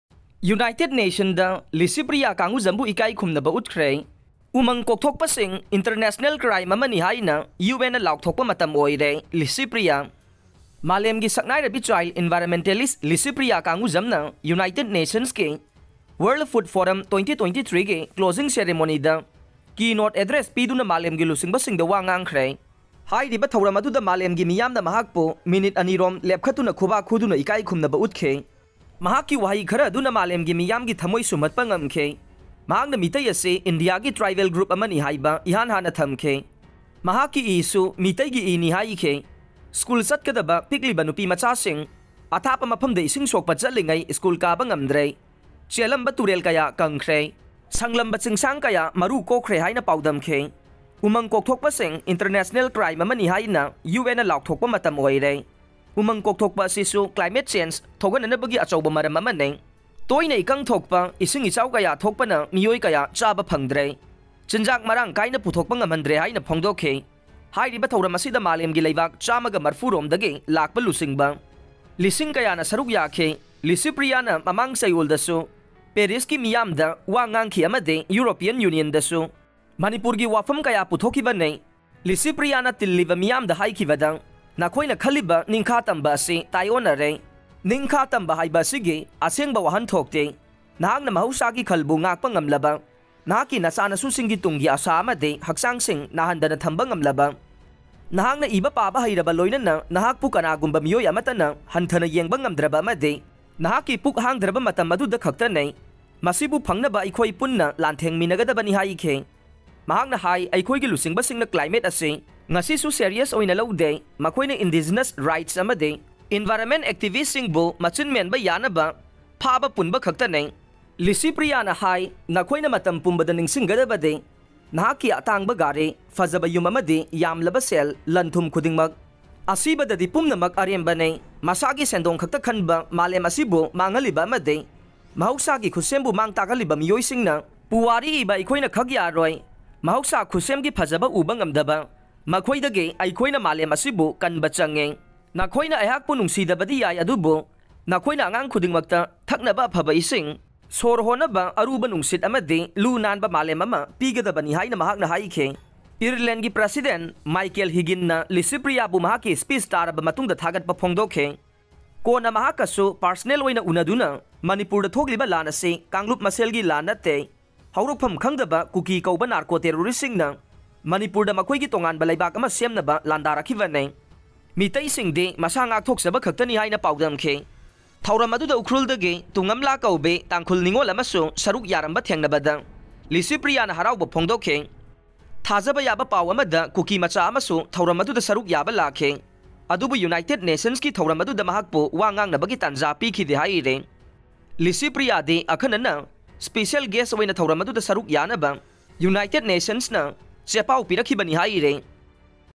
👉United Nations da Licypriya Kangujam bu Ekai Khumnaba Utkhre
👉Umang Kokthokpasing International Crime Amani Haina UN na Laothokpa Matam Oire : Licypriya